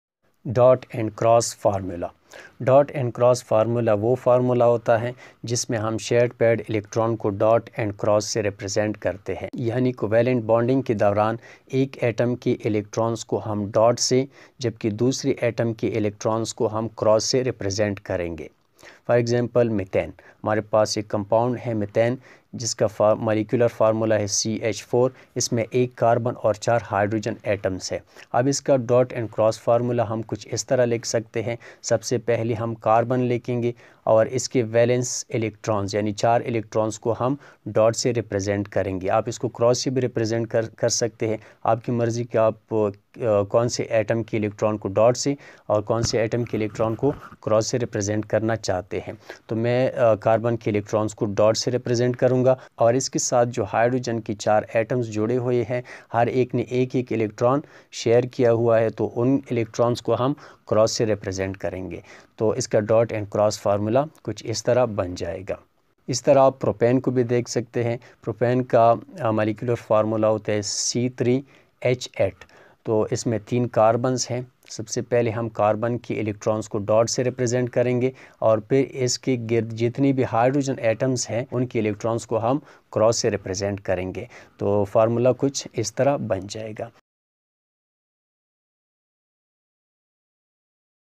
Dot & Cross Formula of Organic Compounds | 10th Chemistry Chapter 11
4. Dot and Cross Formula:
A structural formula in which electrons are shown as dots and cross between various atoms in one molecule of a compound are called Dot and cross formula
or electronic formula.